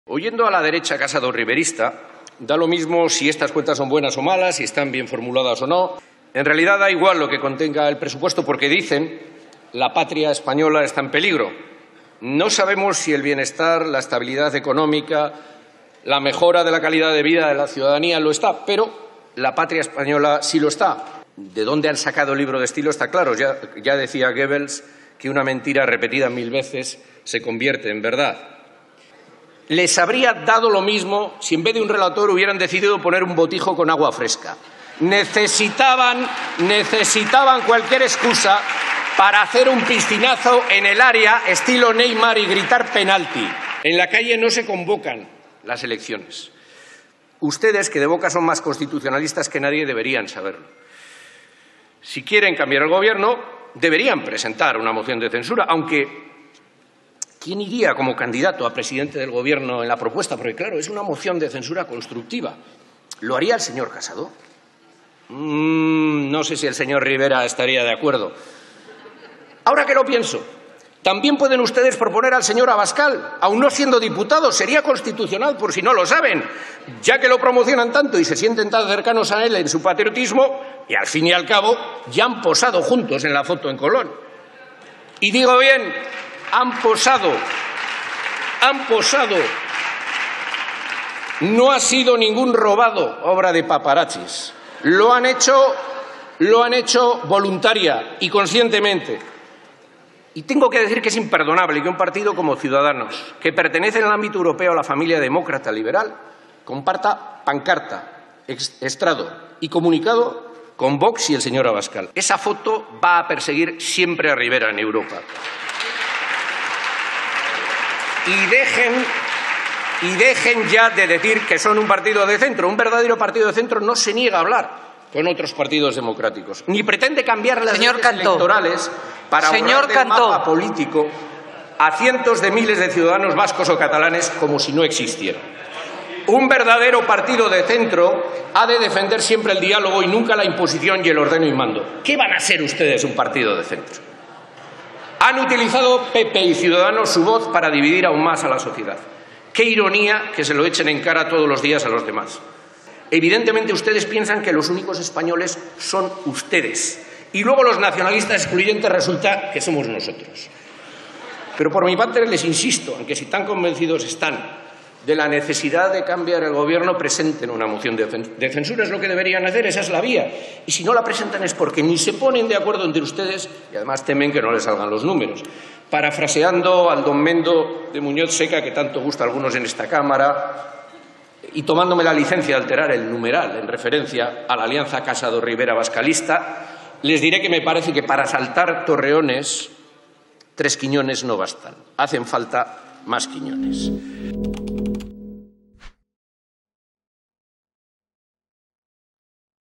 Aitor Esteban: "La foto con Abascal va a perseguir siempre a Rivera en Europa"
El portavoz del PNV en el Congreso recomienda a Casado y Rivera que presenten una moción de censura, incluso que presenten a Santiago Abascal como candidato de consenso.

Apóyanos.